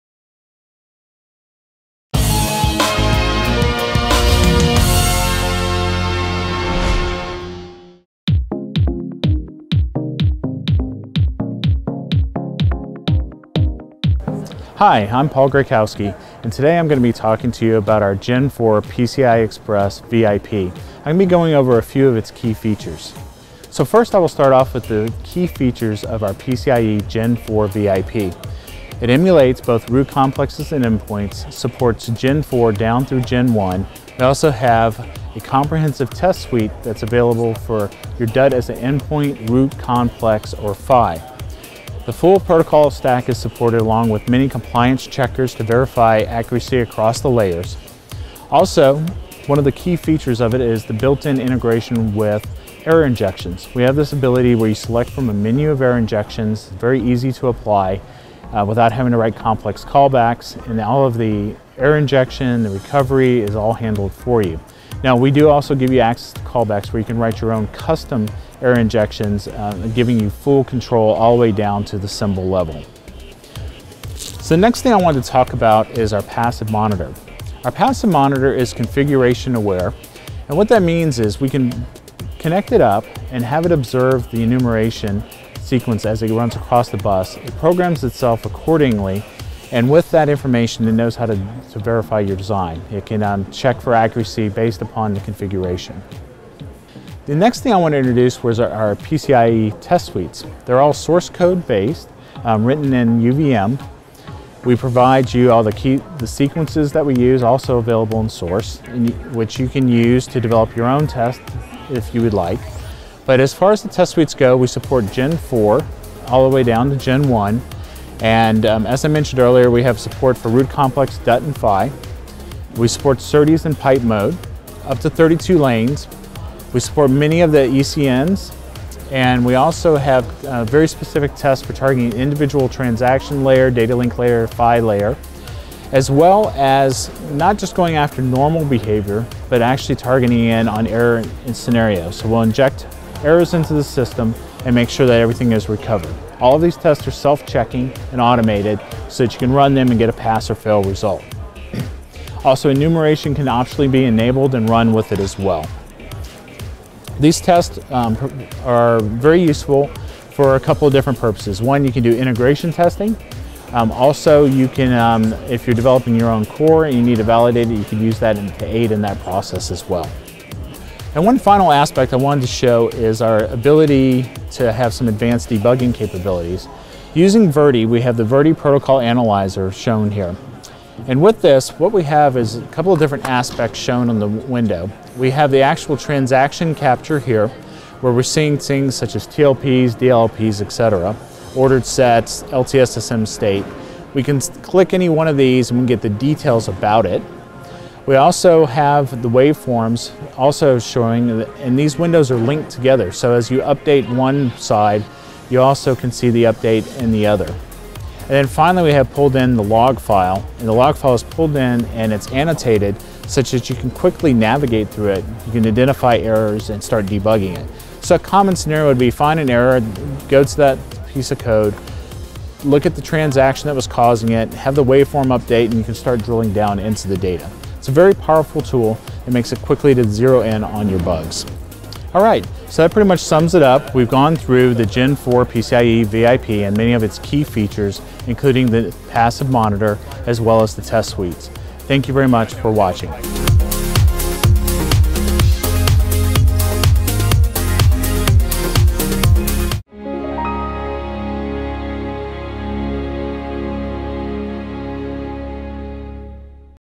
Introducing Synopsys VIP for PCIe Gen4
Here, Paul Graykowski, Corporate Applications Engineer at Synopsys, describes what our Verification IP for PCIe Gen4 can do for you.